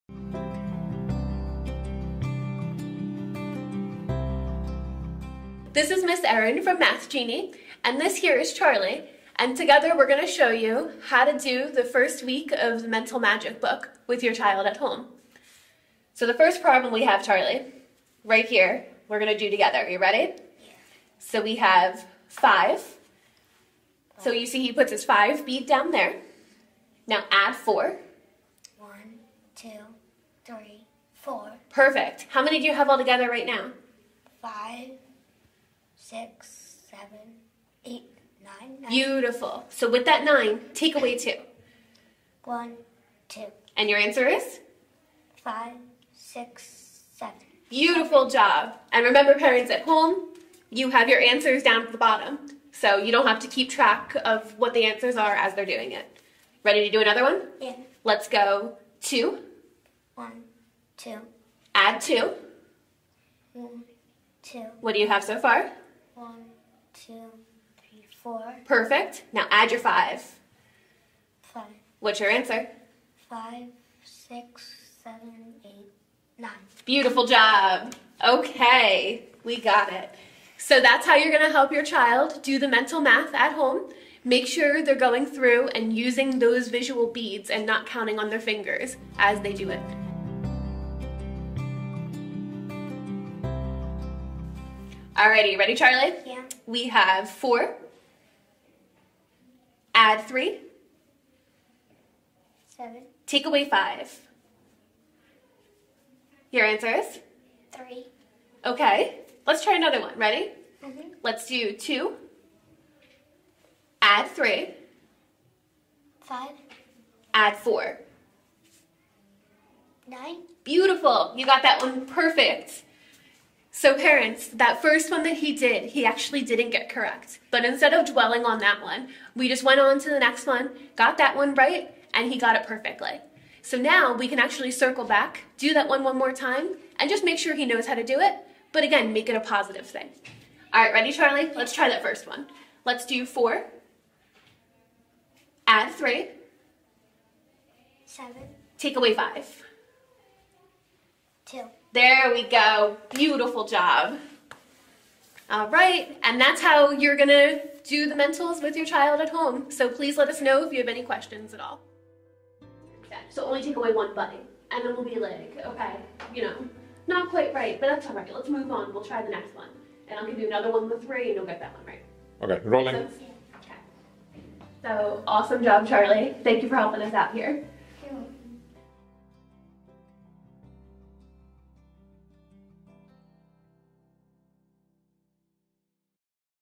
How to do Mental Math practice using the Mental Magic book
This video shows you and your child how to complete Mental Math problems using your Mental Magic Book!

You need to do 5-10 minutes of Mental Math practice per day to develop your child's brain synapses and see results similar to those in this video. Our students have had amazing results by practicing Mental Math every day. It may seem difficult at first, but making mentals part of your daily routine is a sure way to set your child up for success!

Charlie, who is featured in this video, got accepted in New York City's Gifted and Talented program! Take a look at our article featuring Charlie's story:

We find mentals work best when you  make it a game! If your child makes a mistake, go to the next one. You can always come back to the one that is wrong! Remember, daily practice of Visualization of the Abacus will reap huge benefits in the future!